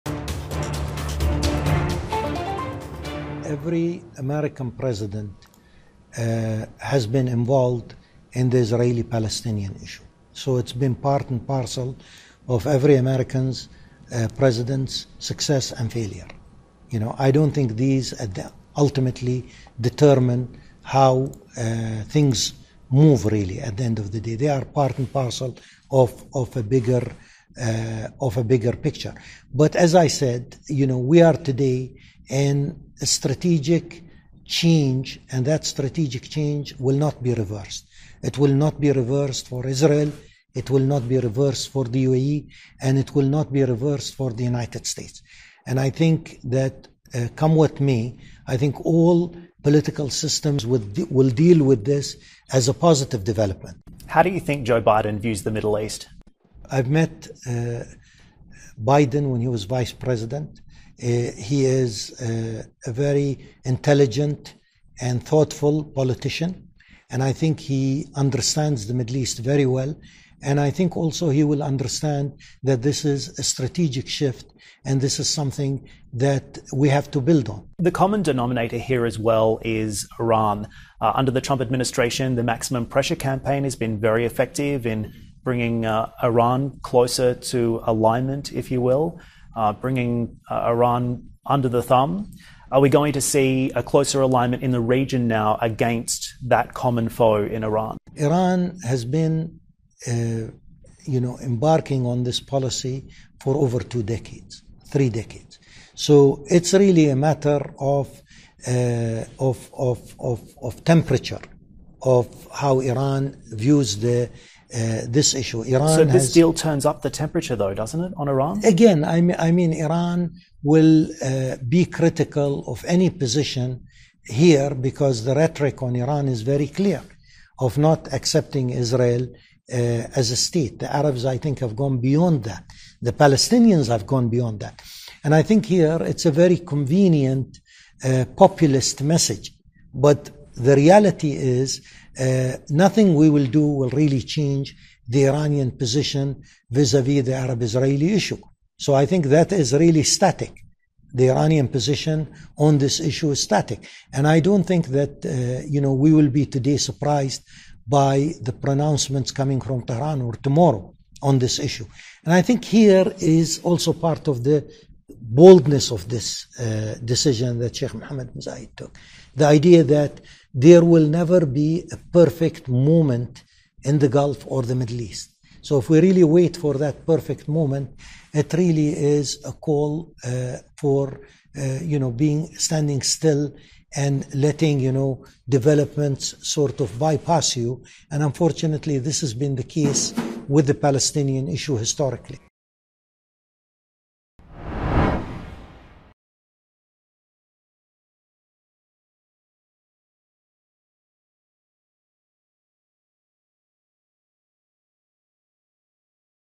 Israel-UAE peace deal won't change Iran's position, Emirati minister says
The U.S.-brokered peace deal between Israel and the United Arab Emirates is an irreversible "strategic shift" in the Middle East, says Anwar Gargash, the Emirati minister of state for foreign affairs.